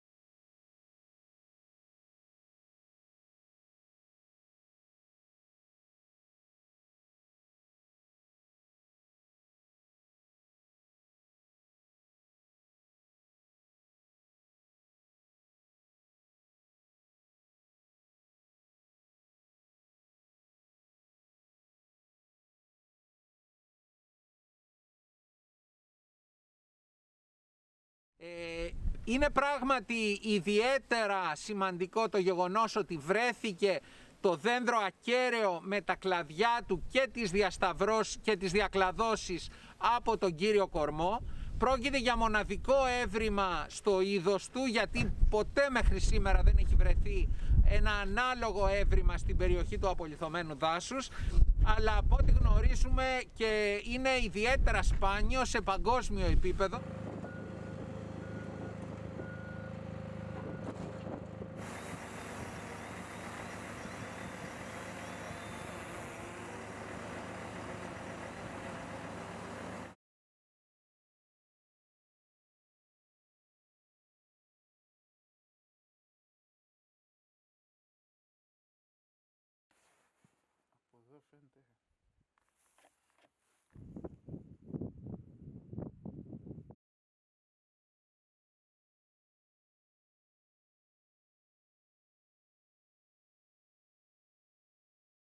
Petrified tree up to 20m years old found intact in Lesbos
Scientists on the volcanic Greek island of Lesbos say they have found a rare fossilised tree. Produced by successive volcanic eruptions, its vividly coloured fossilised trunks are witness to the explosions that buried much of Lesbos under lava and ash between 17 and 20 million years ago. Stretching across almost all of the Greek island’s western peninsula, the petrified forest, a Unesco global geopark, is among the largest in the world.

'Unique' petrified tree up to 20m years old found intact in Lesbos